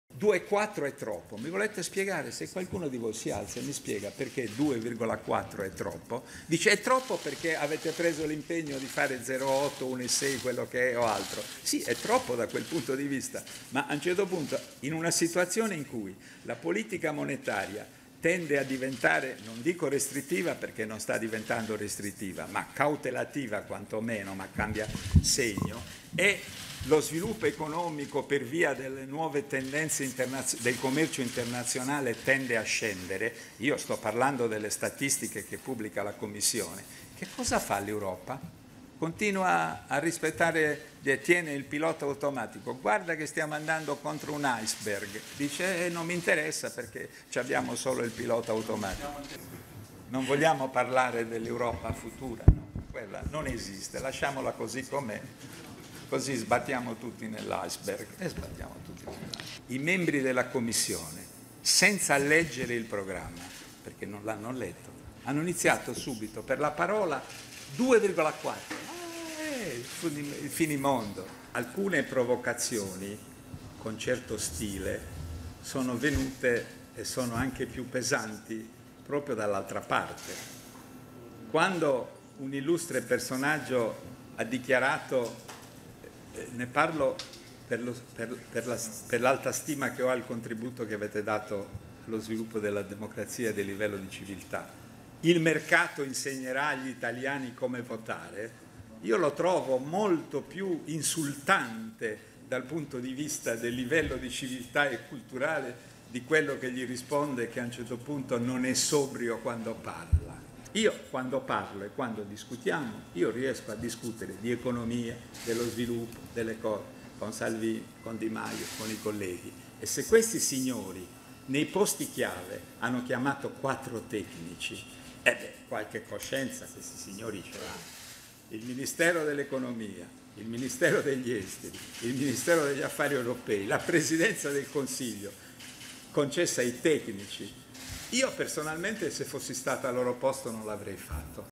Savona: "Se Europa continua con pilota automatico va a sbattere contro un iceberg"
"Cosa dobbiamo fare con una politica monetaria che cambia segno e con il rallentamento della crescita internazionale? In un sistema in cui la politica monetaria sta diventando cautelativa e lo sviluppo economico tende a scendere, l'Europa continua a tenere il pilota automatico", afferma il ministro per gli Affari Europei Paolo Savona alla stampa estera, parlando dei vincoli europei e precisando che è "come se una nave che sa di andare contro un iceberg continuasse comunque ad azionare il pilota automatico"

di Camilla Romana Bruno